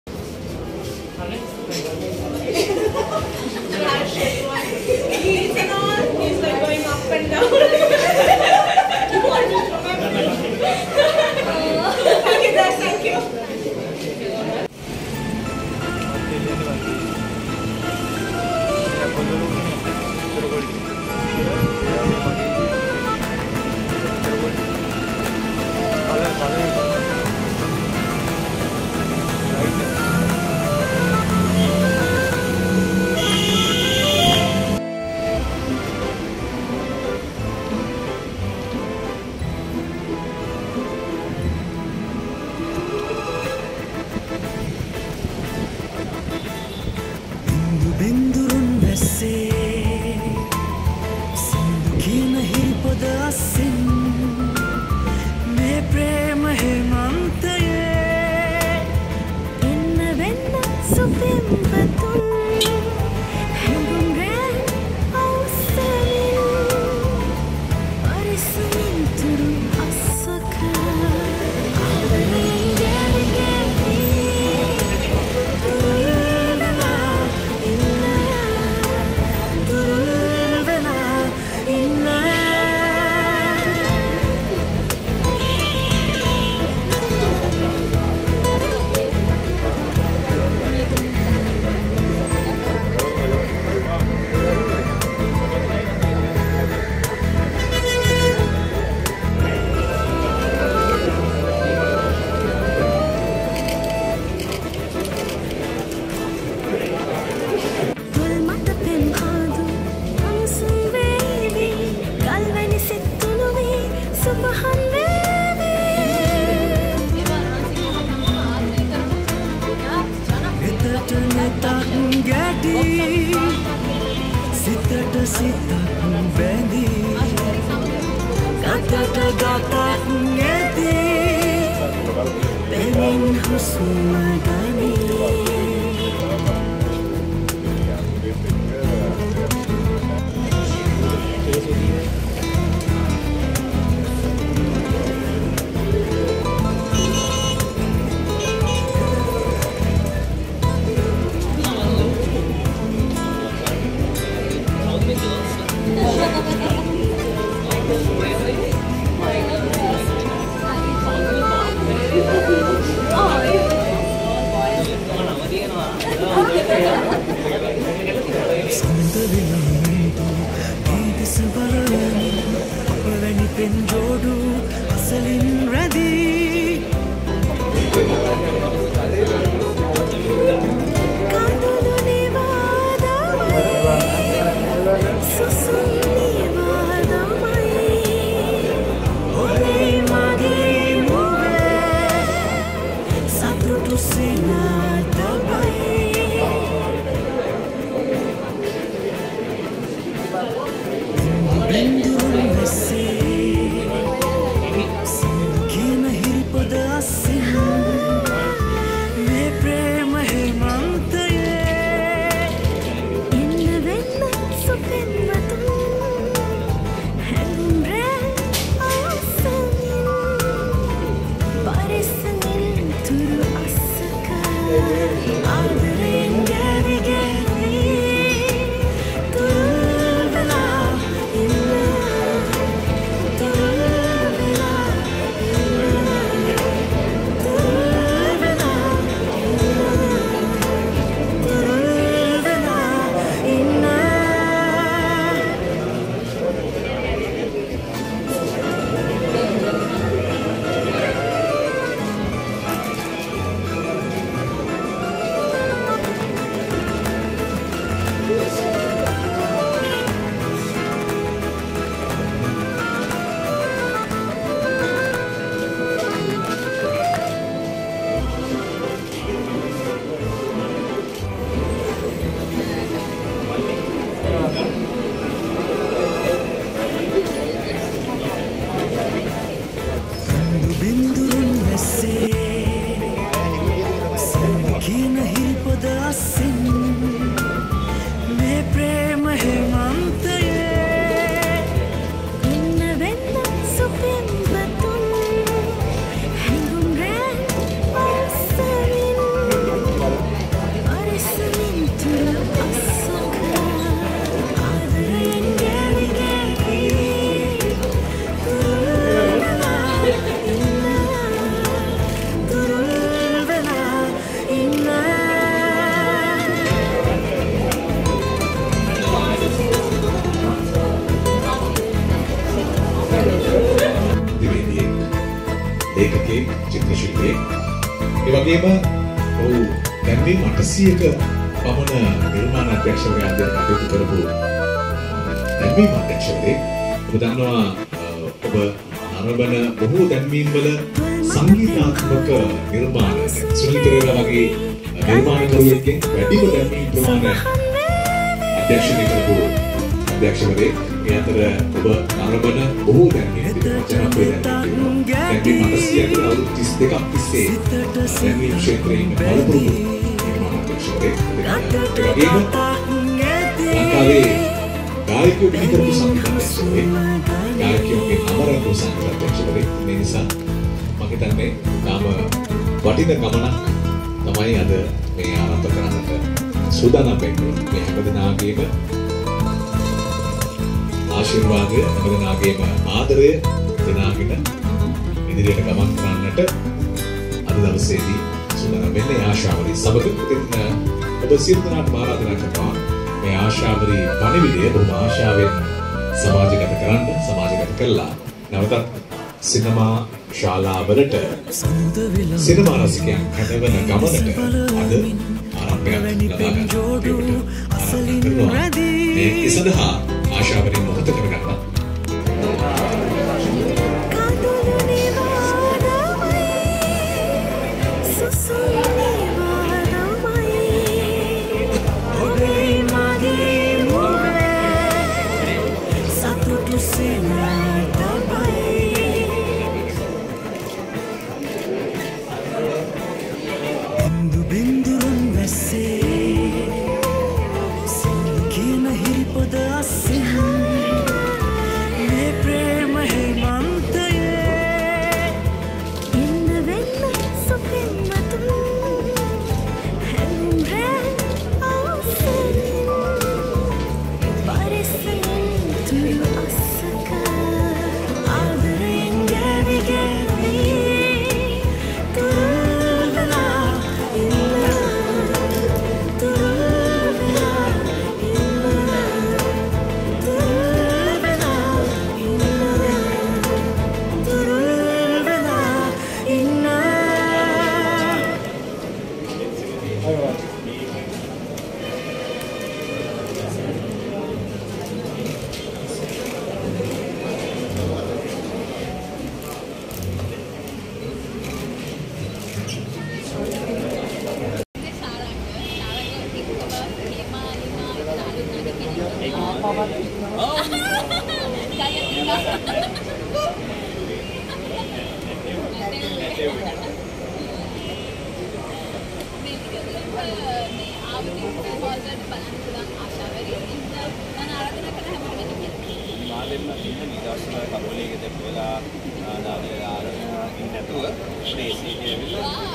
Ashawari(ආශාවරී) - Film Grand Premiere | Hemal Ranasinghe | Ishanka Jahanvi | Saranga Disasekara
Ashawari(ආශාවරී)-  Film Grand Premier
@ Savoy Welawatta

Hemal Ranasinghe
Ishanka Jahanvi
Saranga Disasekara
Ruvi Lakmali
Bimal Jayakodi
Dinithi Walgama